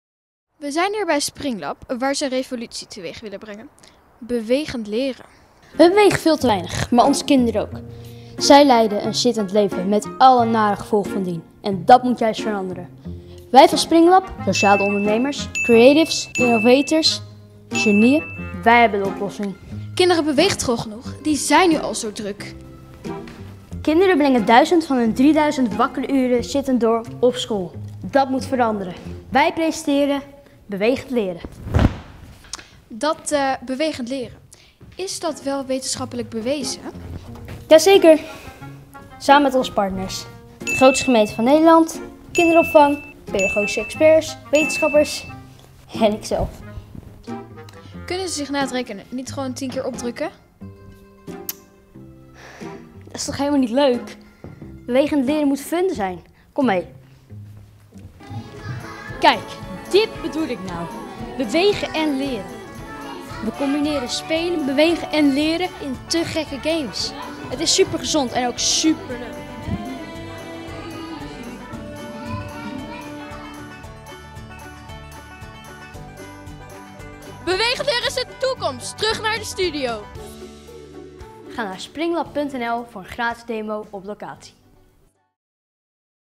BREAKING NEWS: Bewegend leren revolutie in het onderwijs?
Bewegend leren? Is dat wel wetenschappelijk bewezen? En bovendien, kinderen bewegen toch al genoeg? Onze oprichter Jan-Paul de Beer werd onlangs geïnterviewd door een wel heel sceptische journalist. Lukt het hem om haar te overtuigen?